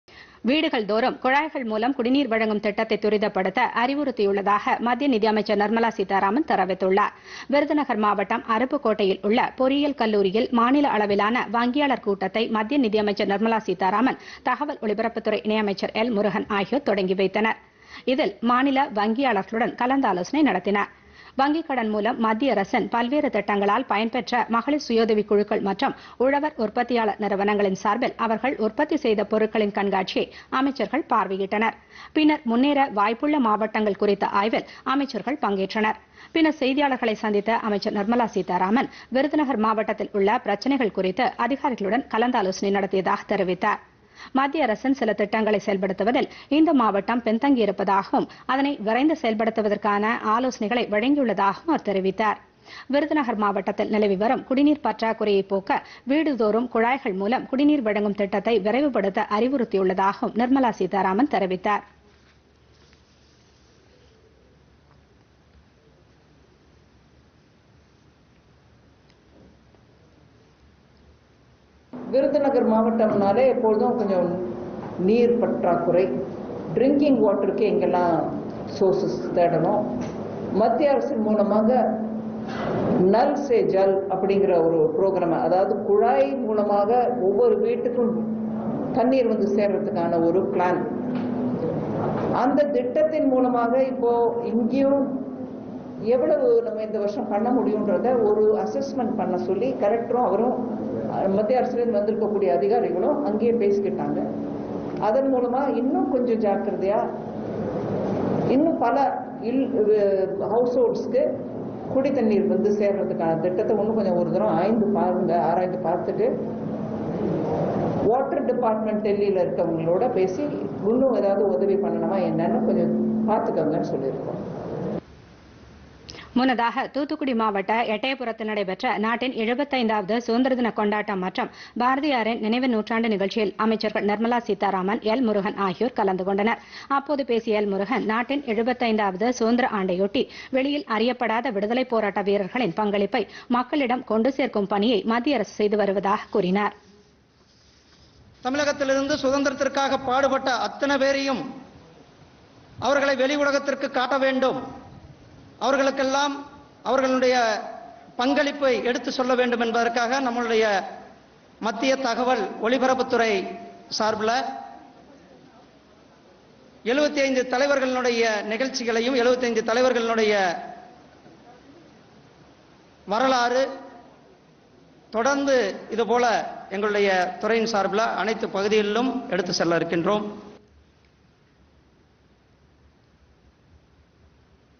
குழாய்வழி குடிநீர் இணைப்பை விரைவுபடுத்துக & தகவல் ஒலிபரப்பு அமைச்சகம் தீவிரம்PodhigaiTamilNews
வீடுகள்தோறும் குழாய் மூலம் குடிநீர் வழங்கும் திட்டத்தை விரைவுபடுத்த வேண்டும் - விருதுநகரில் அதிகாரிகளுக்கு மத்திய நிதியமைச்சர் நிர்மலா சீதாராமன் அறிவுரை

நாட்டின் விடுதலைக்காக போராடிய  அறியப்படாத விடுதலைப் போராட்ட வீரர்களின் பங்களிப்பை மக்களிடம் கொண்டு சேர்க்கும் பணியில் தகவல் ஒலிபரப்பு அமைச்சகம் மும்முரம் - அமைச்சர் எல் முருகன் தகவல்